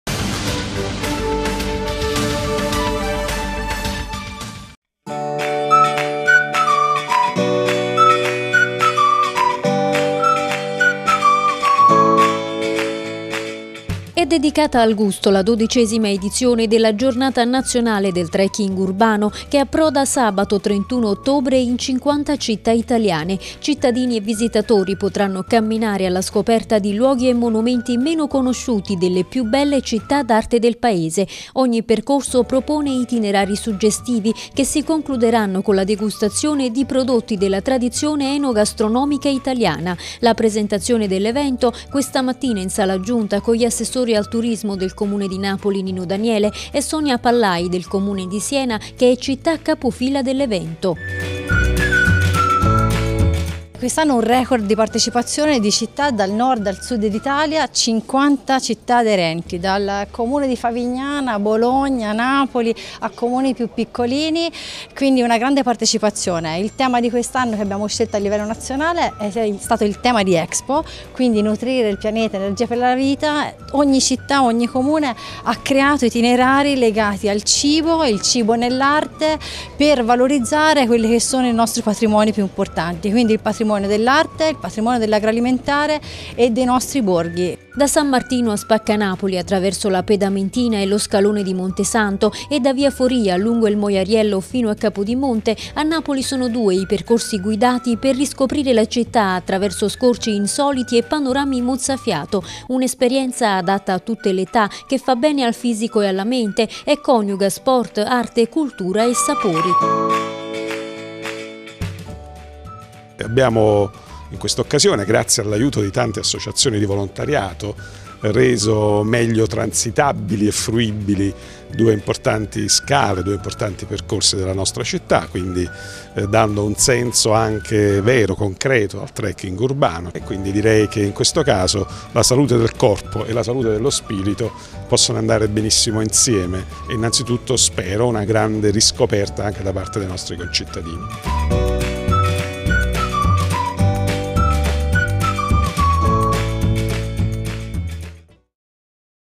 Trekking Urbano, passeggiate e degustazioni per riscoprire la città
Sabato 31 ottobre è la Giornata del Trekking Urbano, in 50 città italiane inediti percorsi guidati alla scoperta delle bellezze e delle tradizioni culinarie del territorio.